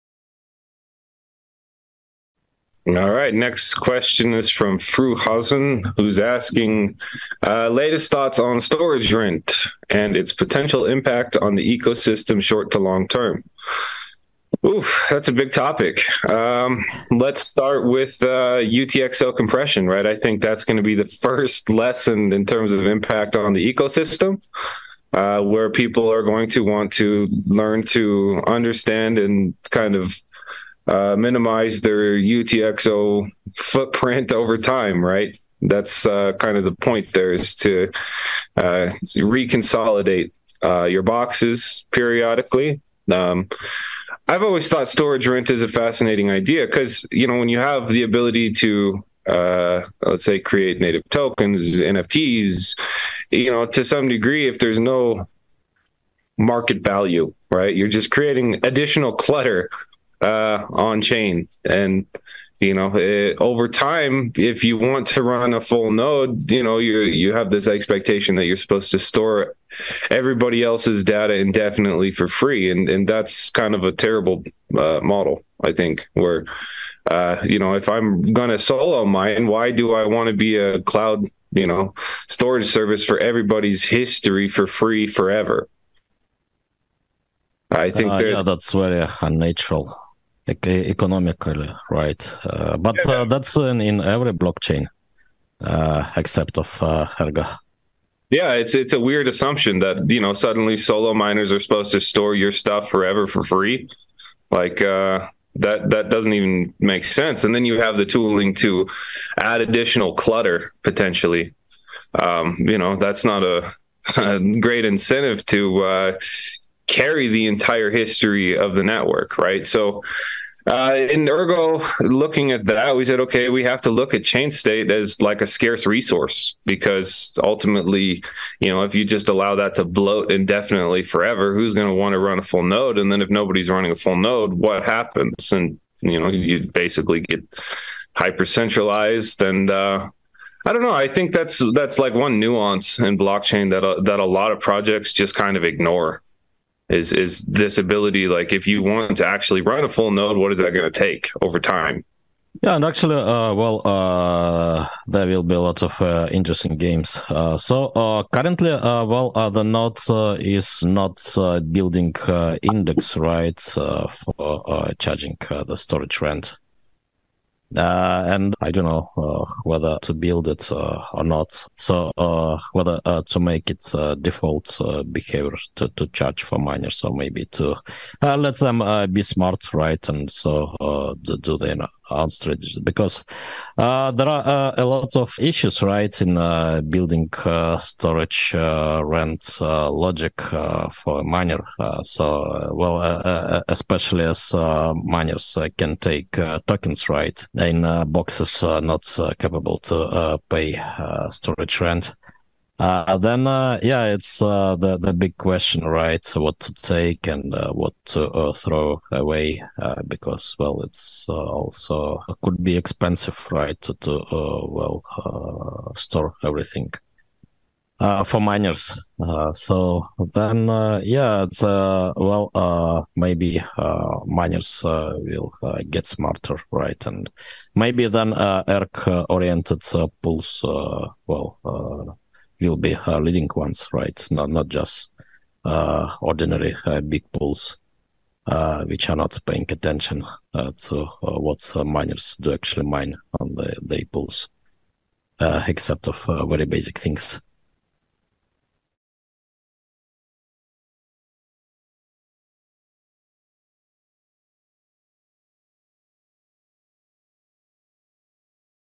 The Benefits of Storage Rent
Alex Chepurnoy and Joseph Armeanio talk about Ergo's Storage Rent feature, and its usefulness in the future.